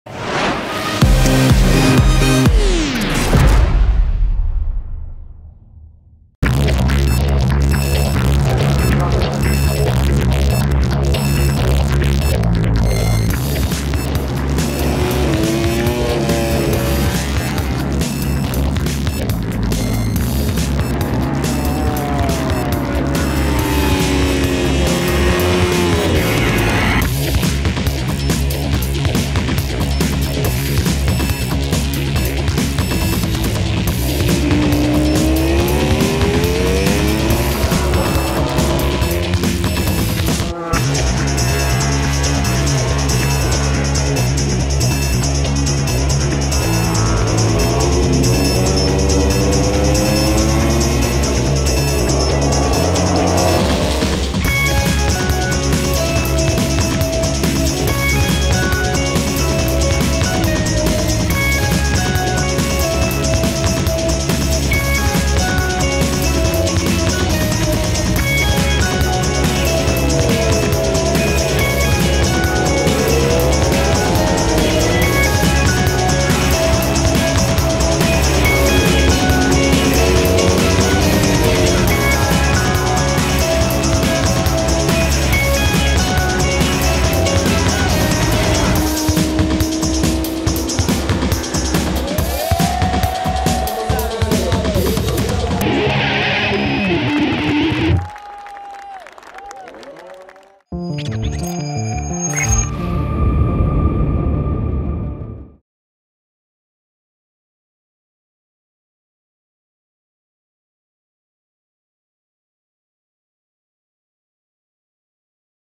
Honda in action: 2018 Gran Premio Octo di San Marino e della Riviera di Rimini
Check out all of the best Honda action from the Misano World Circuit Marco Simoncelli.